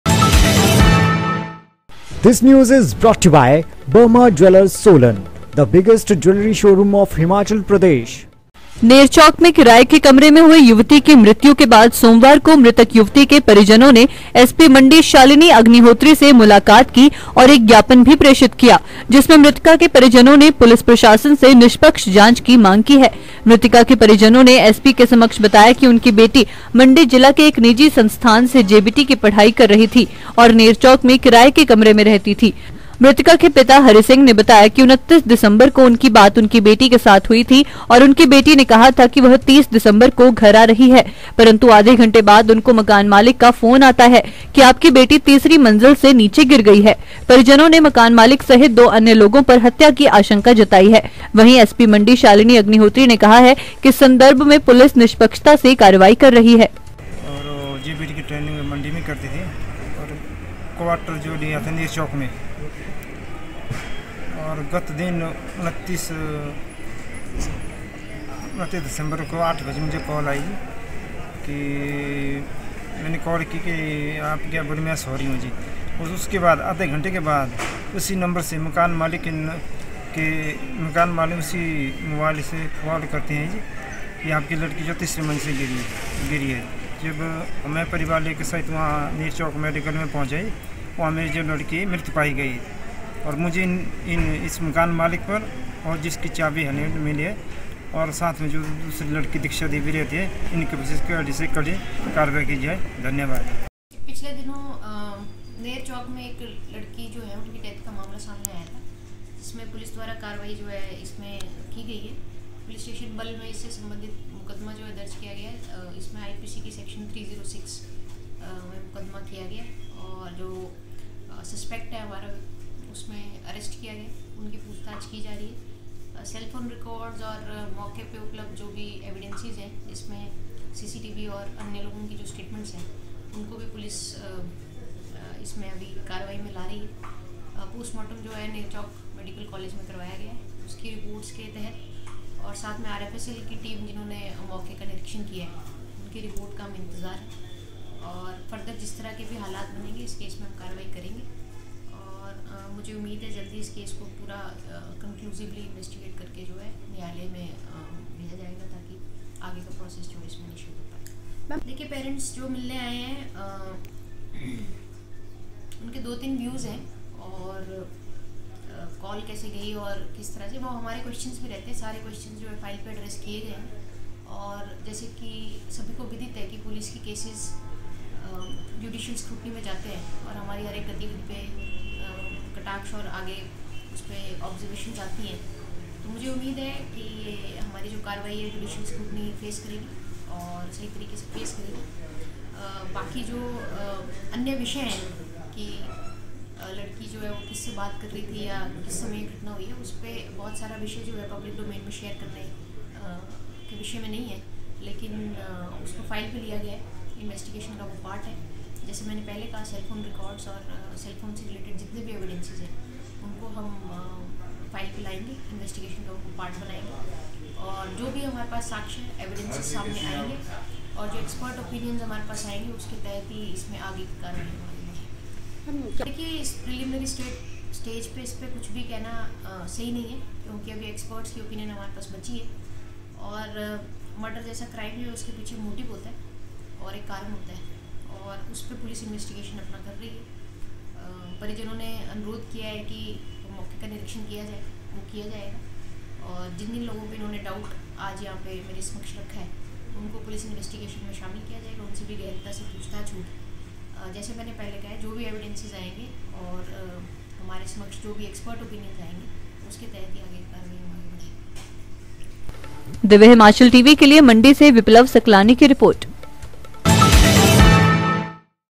परिजनों ने बेटी की मौत की मांगी निष्पक्ष जांच, एसपी को सौंपा ज्ञापन
HimachalNews:
divyahimachaltv
dhtv,tvnews
local news
divyahimachalgoodmorning
khabre ftafat
divyahimachalgoodafternoon